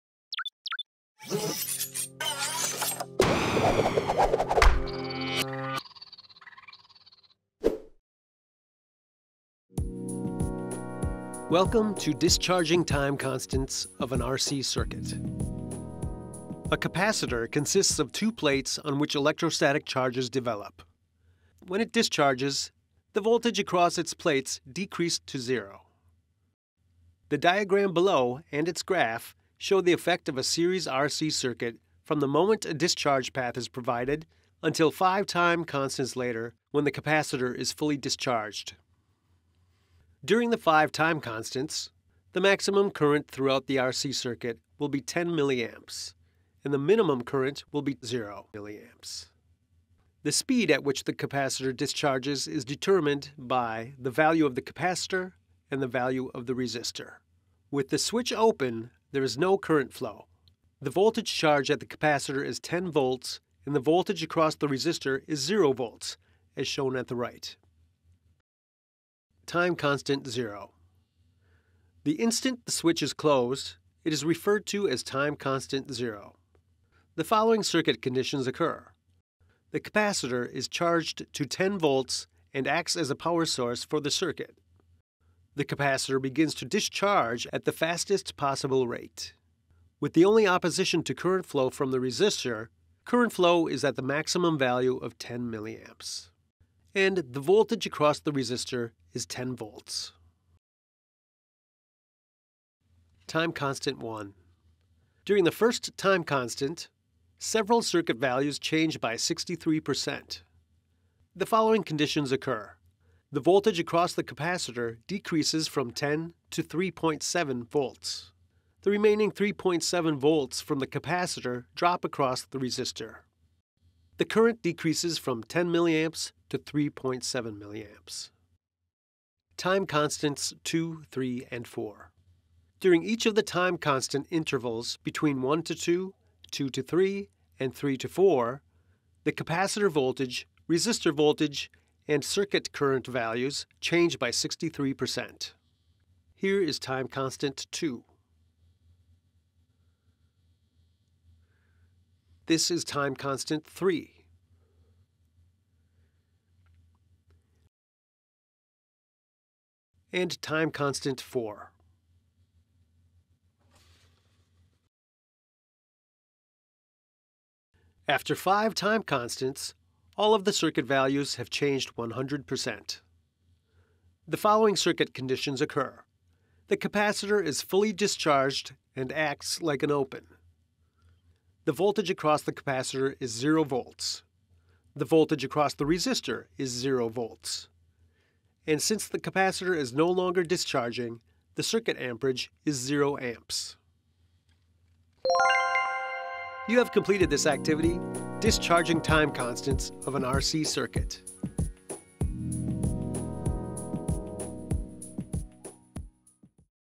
Discharging Time Constants
In this screencast, learners examine how current, voltage and the discharging capacitor of a series RC changes during 5 time constants.

Credits:
    - Content Author: Terry Bartelt
    - Video Developer: Josh Janikowski